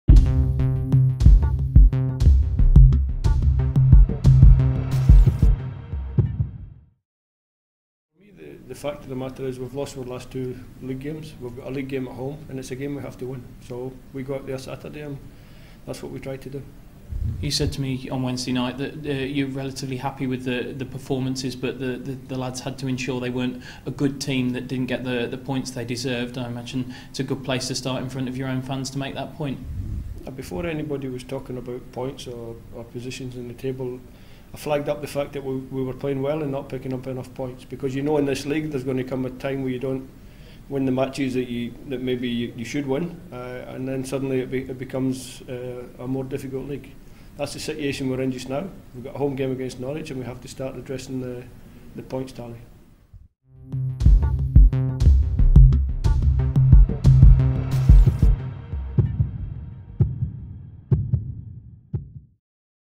Clarke aims to get back to winning ways against Norwich
After back to back defeats against Newcastle and Manchester City, Steve Clarke targets getting back to winning ways against Norwich City on Saturday.